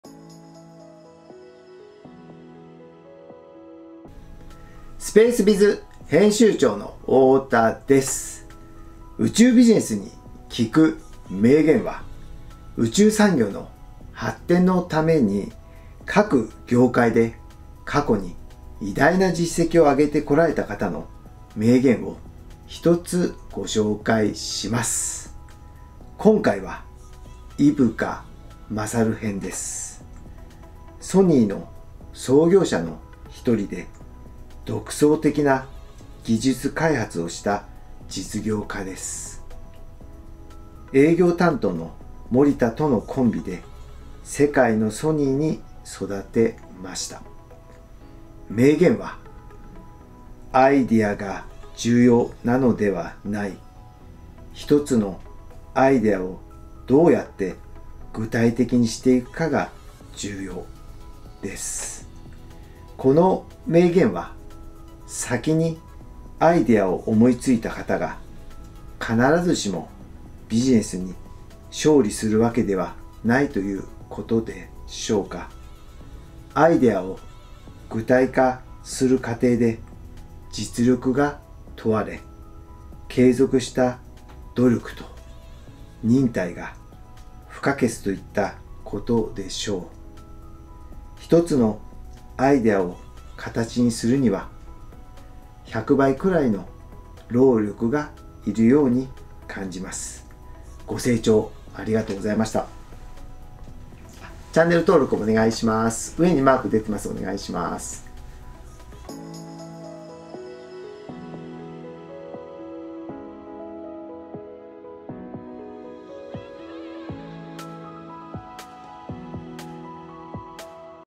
【宇宙ビジネスに効く名言⑮】井深大編
宇宙ビジネスに打ち込む方に、世界の偉人たちの名言をお届けします。
より良い未来へ前進する活力になればと思います。

■Space Bizアカデミー（宇宙ビジネスセミナーをやってます）

■Twitter（宇宙を主軸にビジネス全般の注目情報を発信しています）